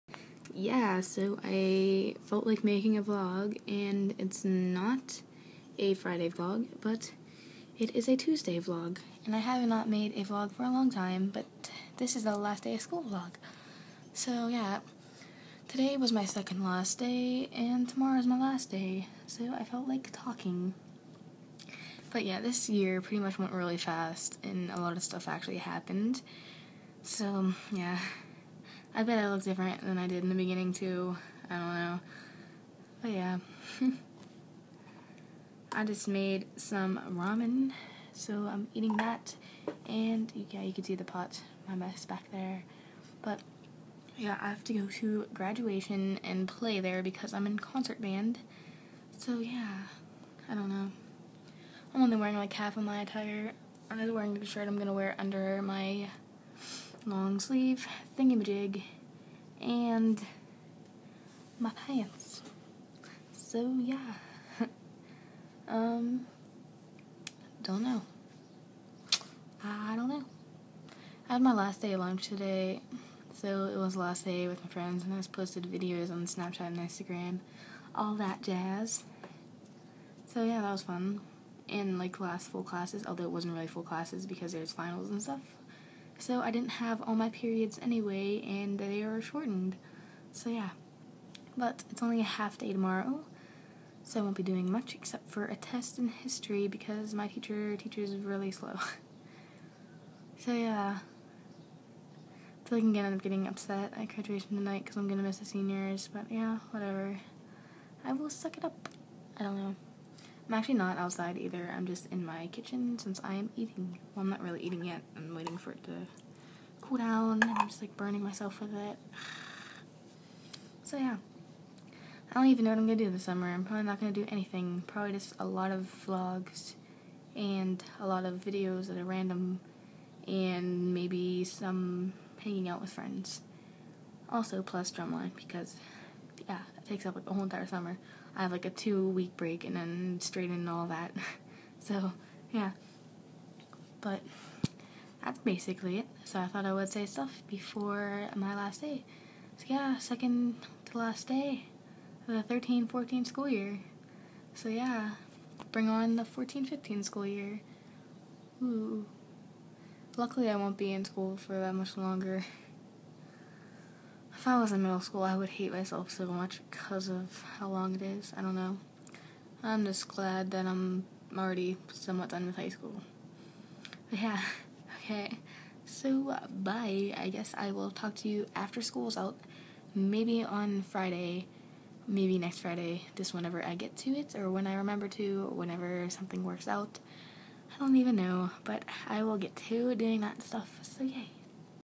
Old last day Vlog
Vlog from the last day of school. Never got to upload until now.